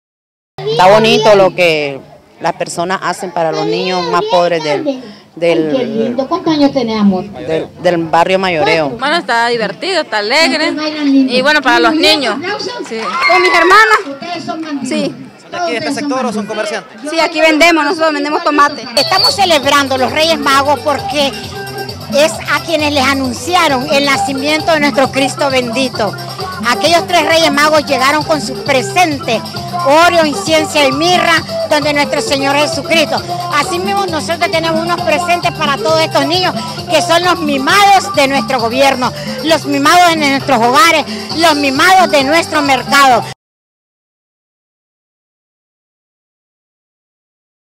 Comerciantes nicaragüenses celebran el día de los reyes magos.
comerciantes  nicaragüenses celebraron este domingo el Día de Reyes, última celebración navideña que según la tradición católica tiene que ver con la visita realizada por los Reyes Magos al niño Jesús en la pequeña ciudad de Belén hace más de dos mil años.